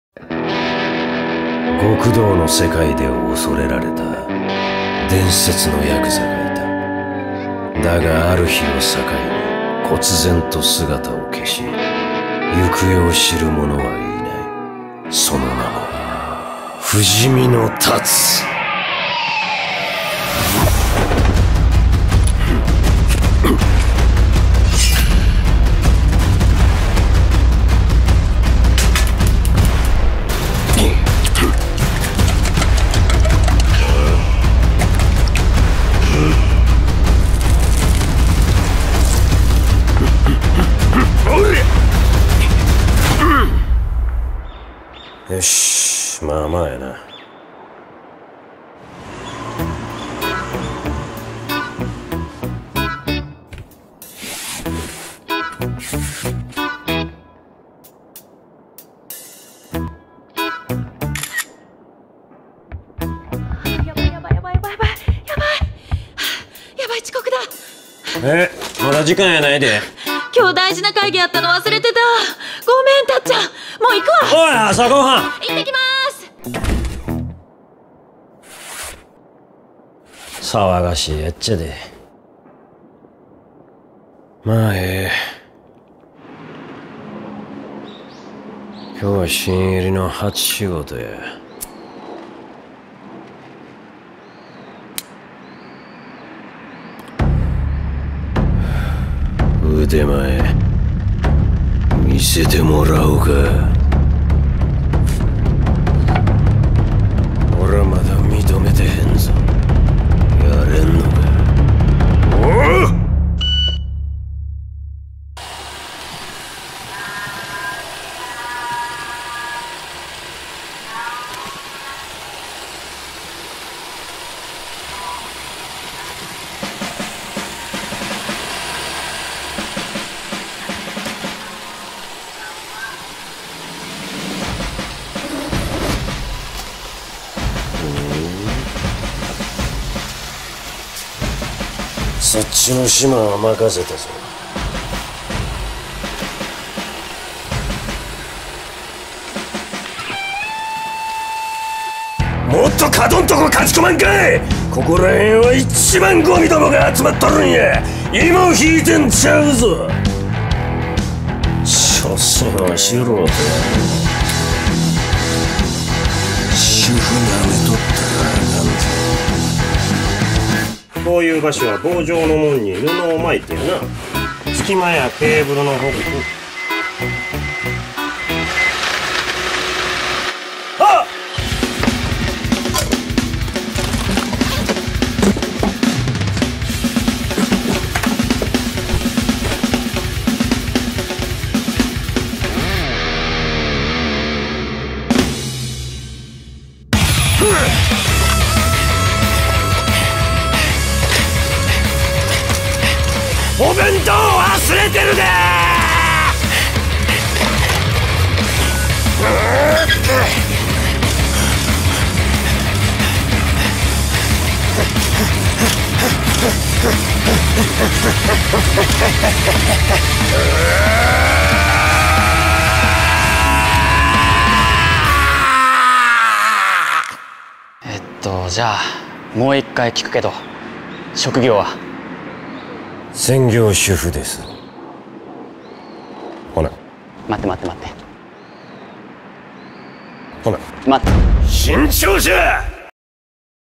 La Voie du tablier - Vidéo Promotionnelle (Live Action)
Vidéo promotionnelle (Live Action) du manga "La voie du Tablier" de Kousuke Oono. Elle célèbre les 1.2 millions de tomes en circulation du manga, avec le célèbre acteur et seiyuu, Kenjiro Tsuda dans le rôle de Tatsu l'immortel.